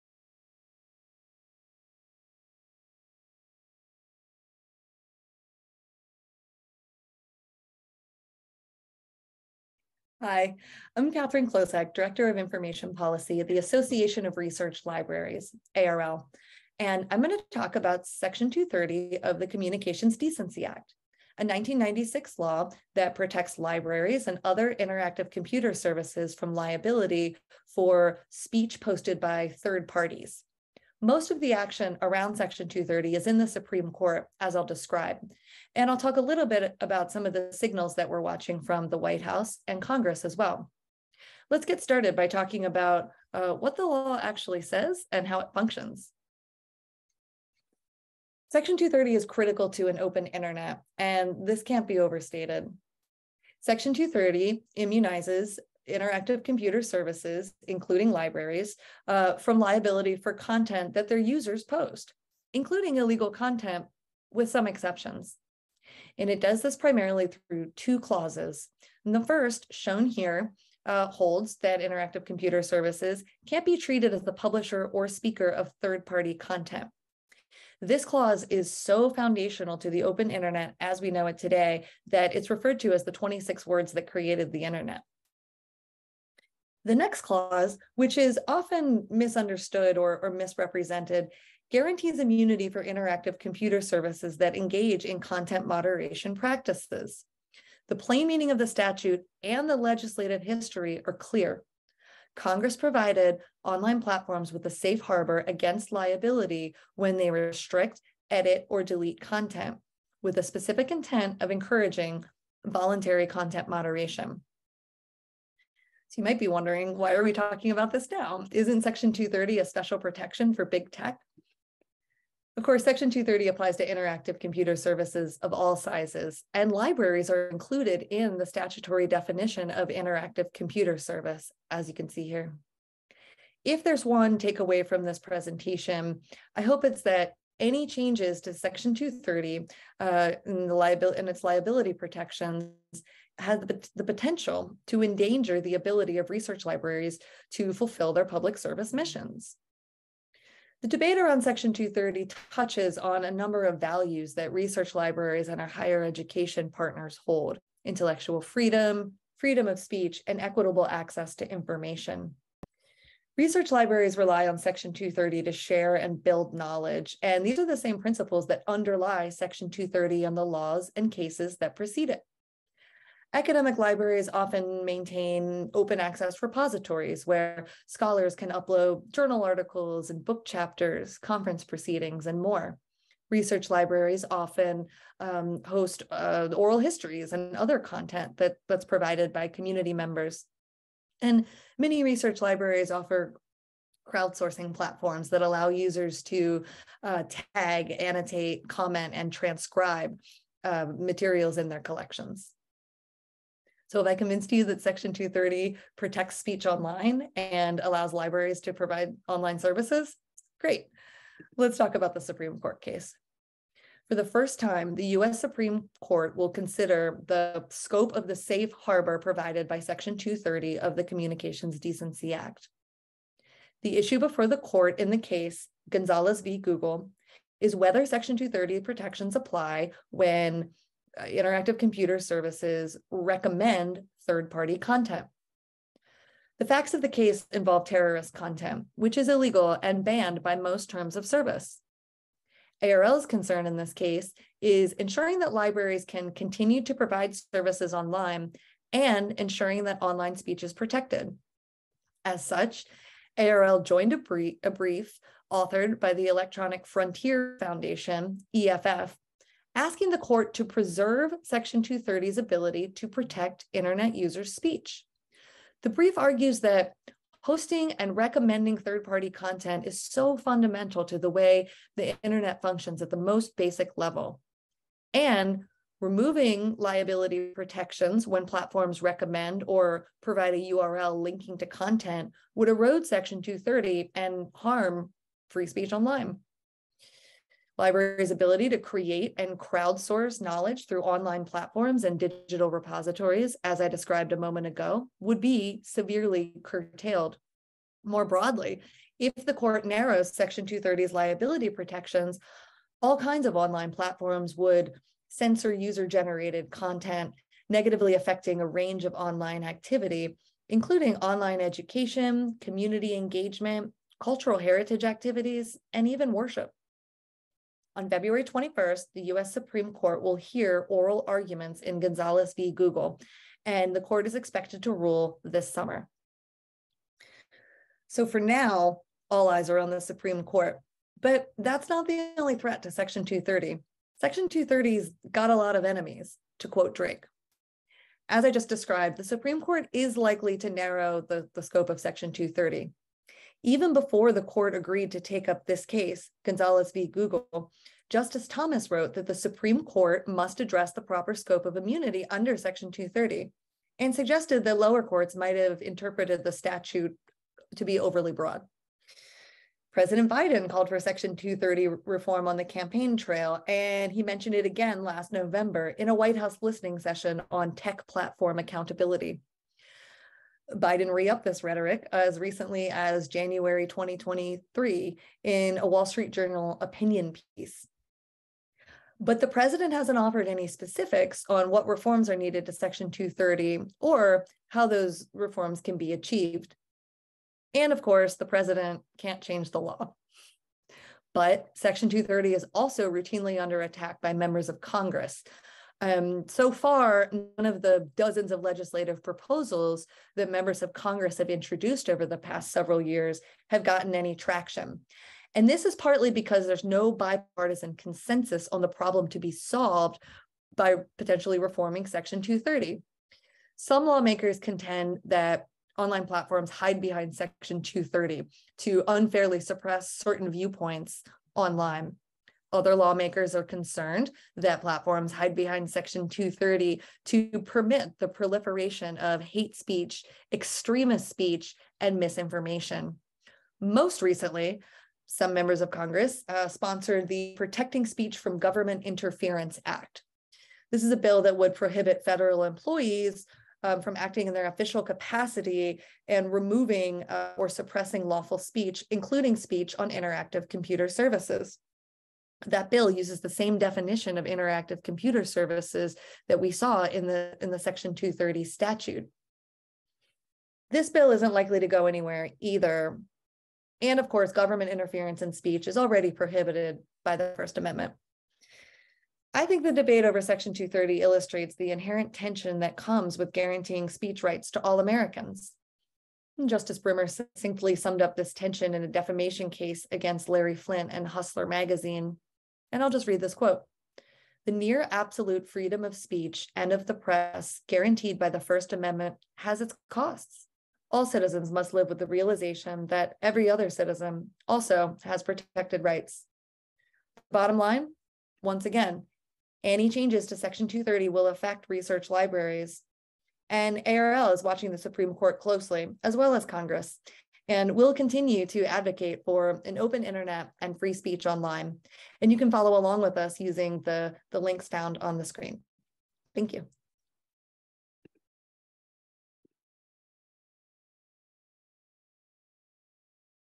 Research Libraries and Section 230
Katherine Klosek, Association of Research Libraries

In this briefing, ARL's Director of Information Policy Katherine Klosek will give an overview of Section 230, a 1996 law that protects libraries and other internet service providers and users from liability for speech shared by third parties and content moderation practices. Klosek will highlight specific ways that libraries rely on the statute's protections and give an overview of what to expect as the 118th Congress eyes changes to this fundamental law. The briefing will also cover Gonzalaz v. Google—the first US Supreme Court case on Section 230—and an amicus brief that the Association of Research Libraries joined in support of the law's protections.

CNI Pre-Recorded Project Briefing Series: February 2023